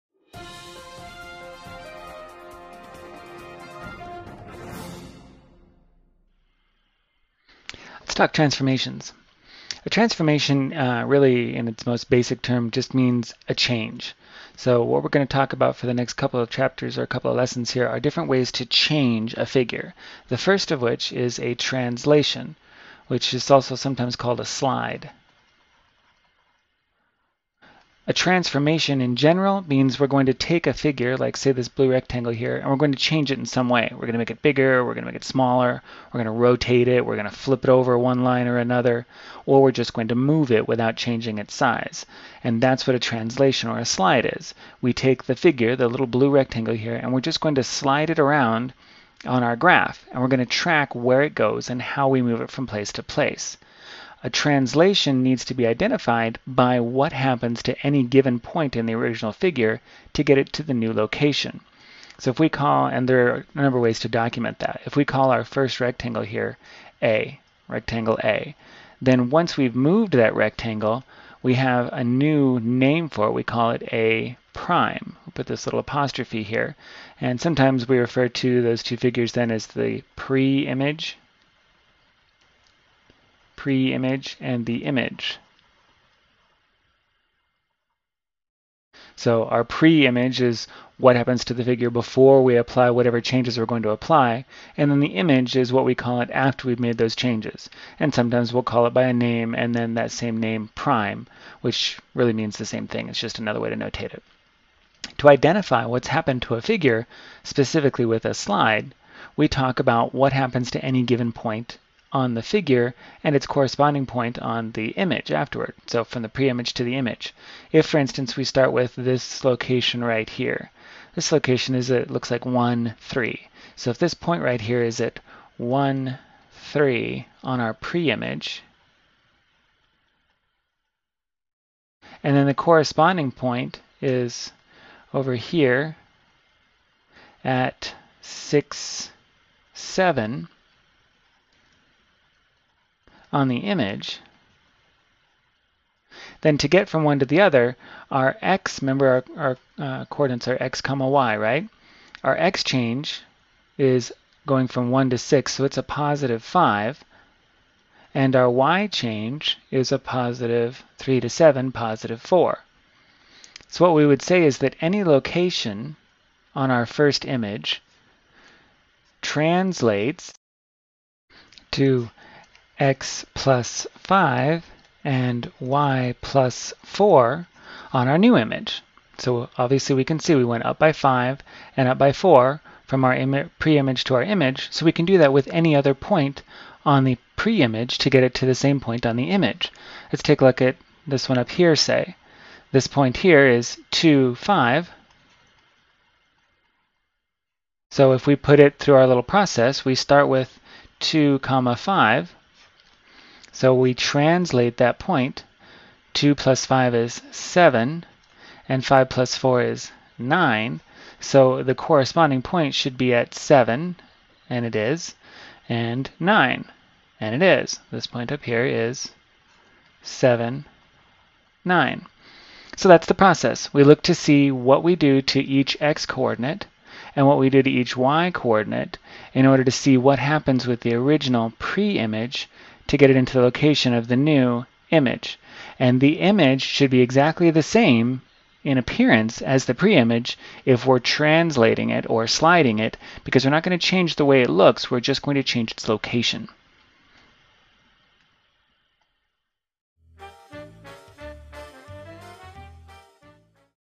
Transformation - Translation: Lesson (Basic Geometry Concepts)
Here you'll learn what a translation is and how to find translation rules.

This video provides the student with a walkthrough on translation.